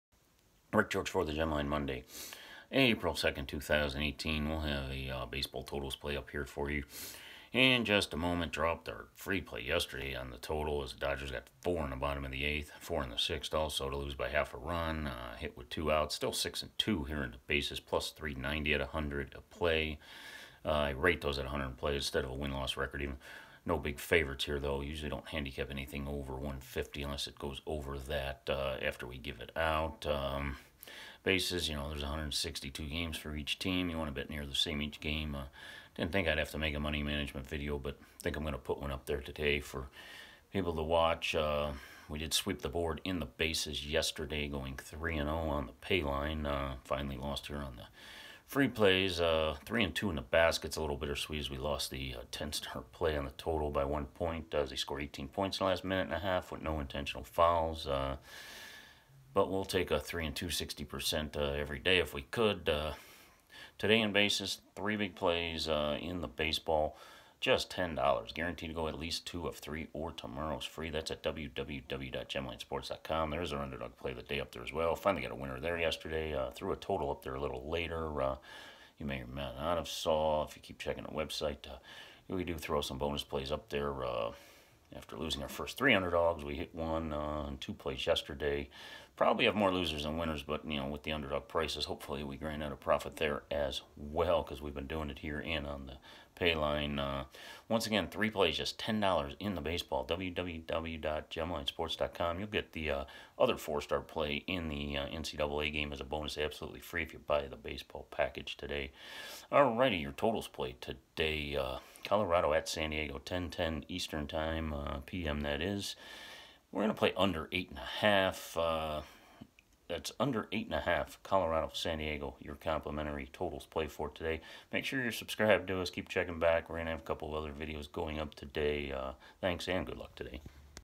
FREE MLB TOTALS pick 04/02/18 late game
Get a free MLB pick from handicapper Rick George of the Gemline. This play is an over/under play in late action.